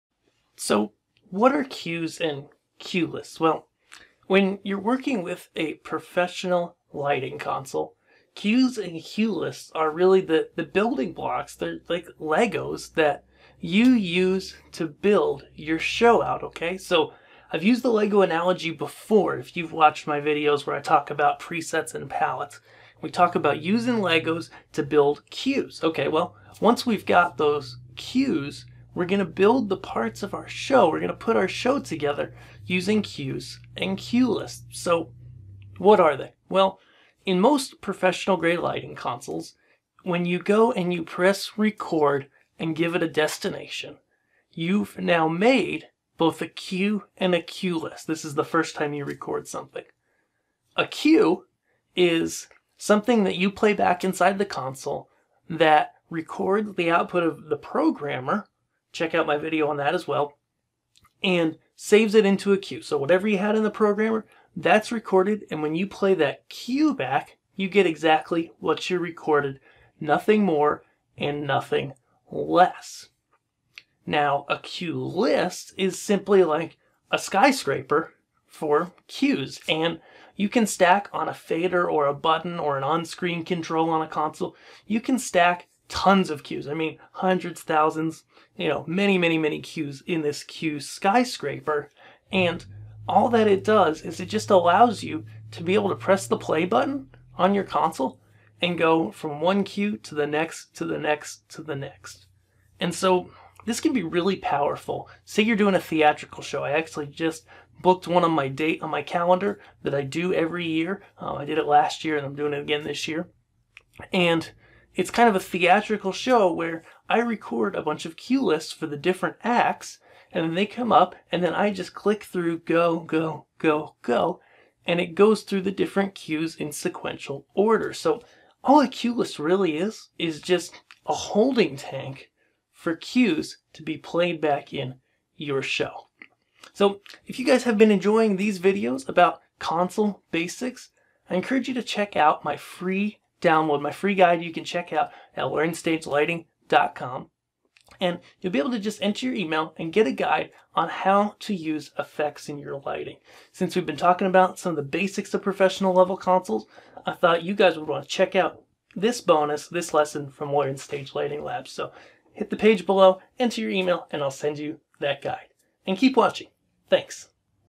What are Cues and Cuelists? - Professional Lighting Console Basics
How do cues and cue lists relate, and how do you create and work with them in a lighting console?

Video Card: GTX1050Ti

Pixels Driven By ENTTEC Pixel Octo Monetization ID TGG0OCXYH5ACR9QY